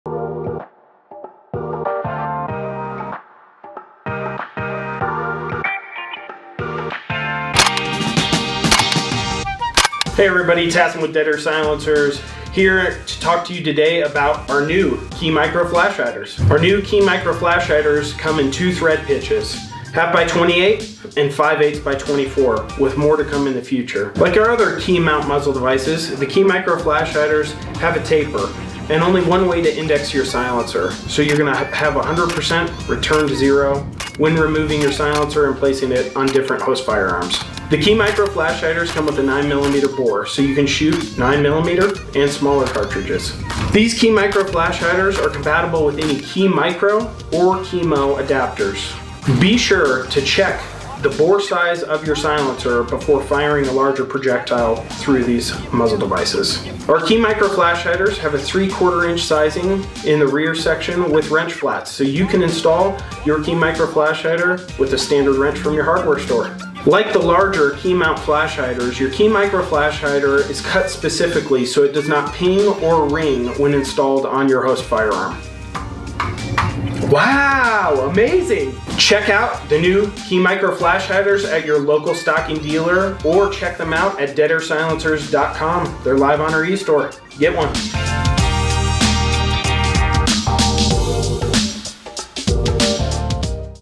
KeyMicro Flash Hider video
In the market for a new flash hider? Take a look at the ALL NEW KeyMicro flash hider from Dead Air Silencers. Compatible with the KeyMo mounting system, this smaller version of its older counterpart has the same features while offering a smaller profile to your host.